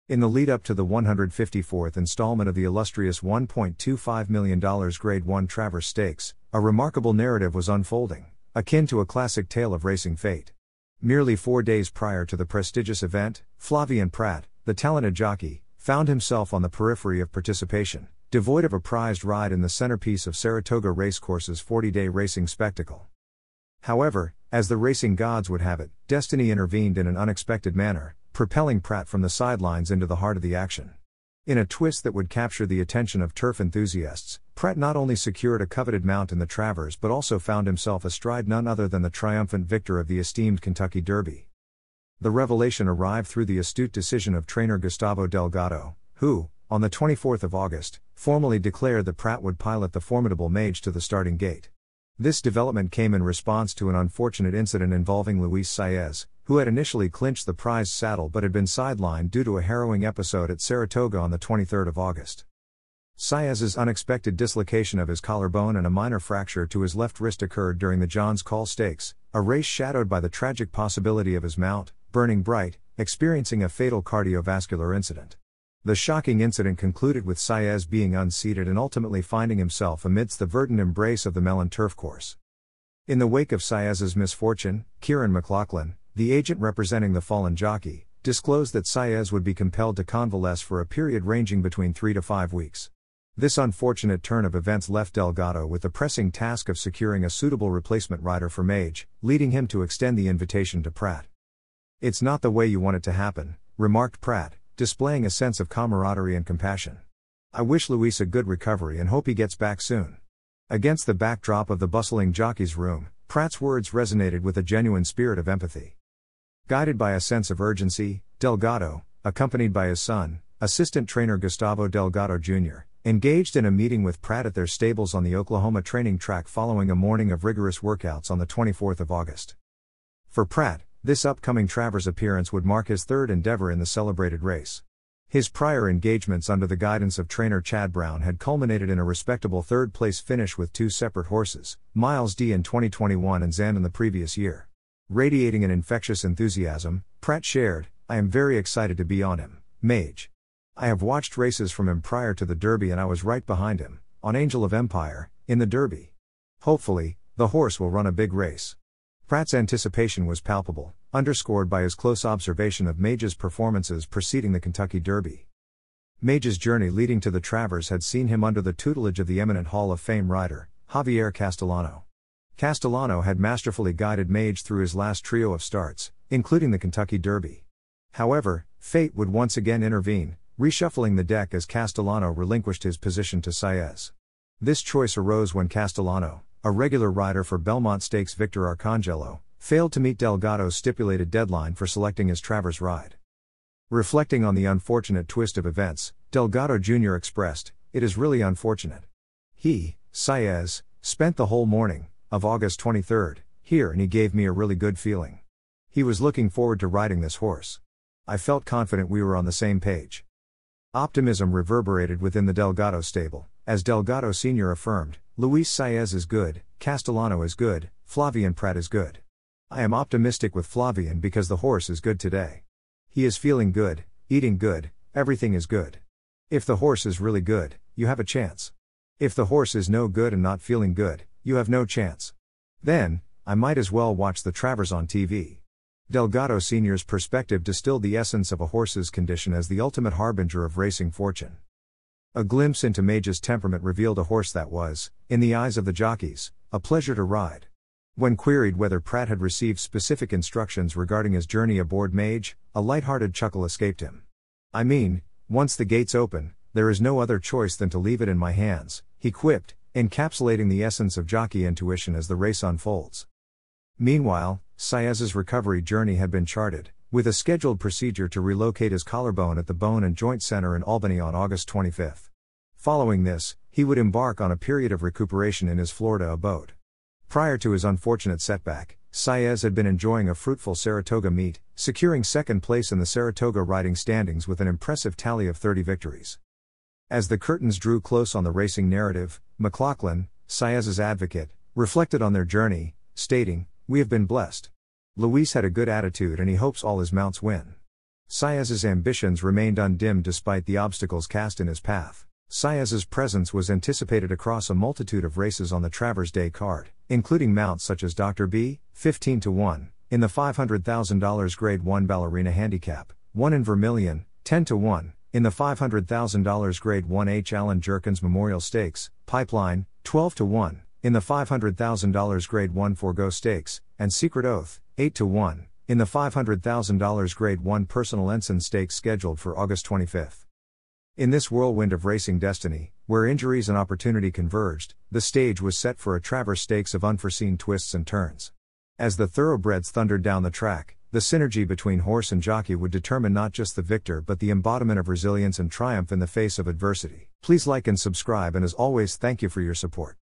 Prat Steps in: Travers Stakes Fate Shifts Amid Saez's Injury
"Flavien Prat replaces injured Saez as jockey for Mage in the Travers Stakes after a collarbone injury. Saez's journey, Delgado's optimism, and Saez's other mounts on Travers Day noted."